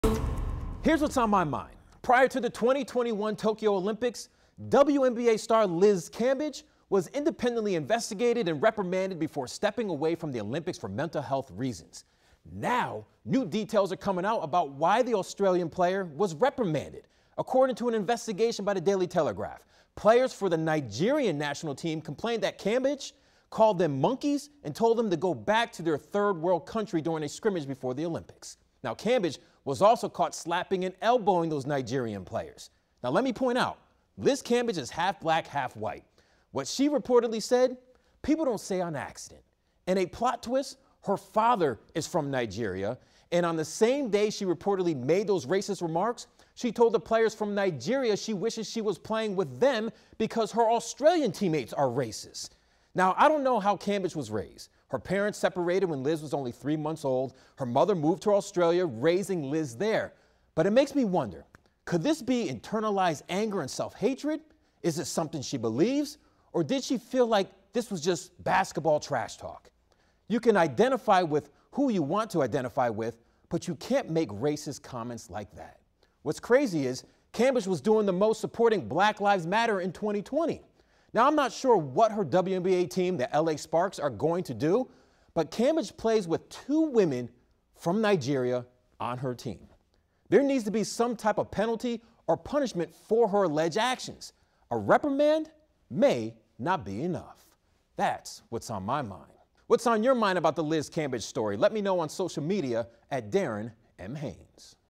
Report: WNBA star Liz Cambage accused of racial slurs
WUSA9's Darren Haynes discusses the news about the women's basketball star.